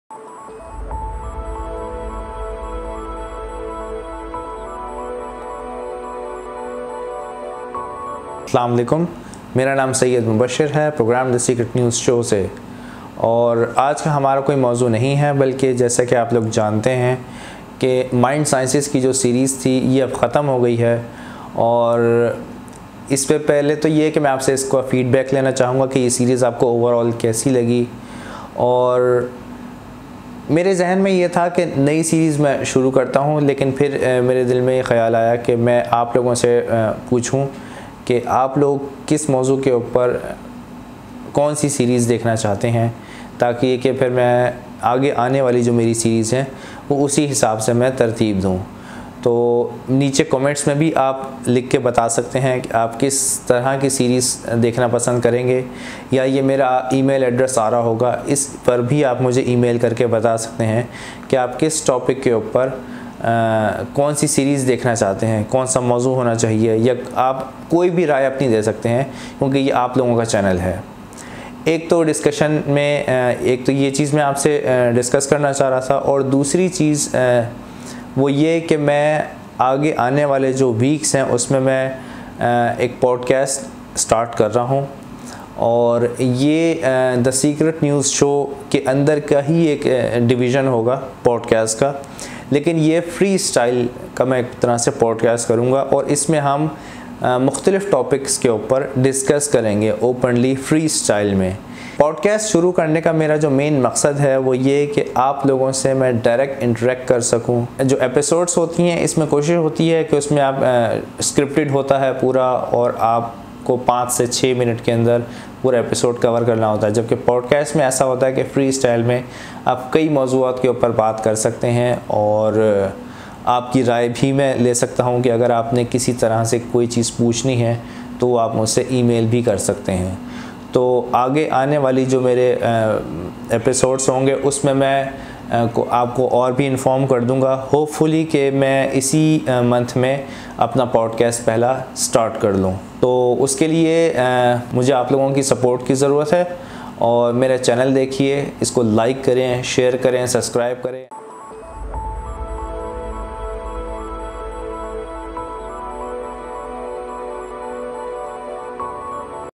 The Secret News Show Update and Podcast Announcement
The Secret News Show planning to start a Podcast and New series. Podcast main topics are Secret Space Program , Cosmic Hisotry , UFO's , Aliens and Cosmic Mysteries.